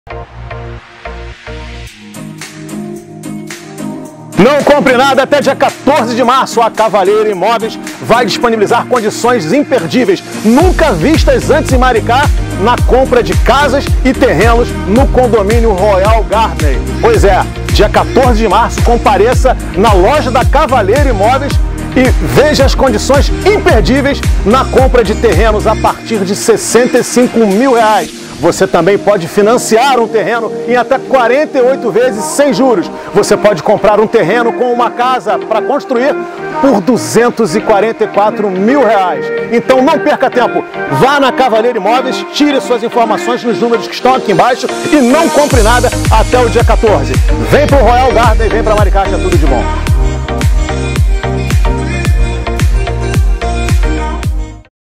Convite para nosso Evento dia 14/03, na Cavalleiro Imóveis Maricá
📣 Atenção quem mora ou quer morar em Maricá - RJ. Vai rolar um GRANDE EVENTO da Cavalleiro Imóveis,  com a venda dos últimos (sim, últimos) lotes do condomínio Royal Garden. Vai ser no dia 14 de março, com tabela sem juros. Teremos lotes com SUPER DESCONTO À VISTA, financiamentos com entrada reduzida ou parcelada, possibilidade de usar 🚗 carros e🛵  motos como forma de pagamento e muitas oportunidades para você vir morar em Maricá!

🏡Teremos casas na modalidade "aquisição de terreno e construção" com financiamento da CAIXA  no Condomínio  e também casas à vista com desconto. Essa é a sua grande chance de mudar de vida. Não perca!

✨ Guarde bem essa data:
📍 14 de Março, de 9h às 17h
🔎 Rodovia Ernani do Amaral Peixoto, nº28354, Km 28, Lj 1, 2 e 3 - Centro, Maricá.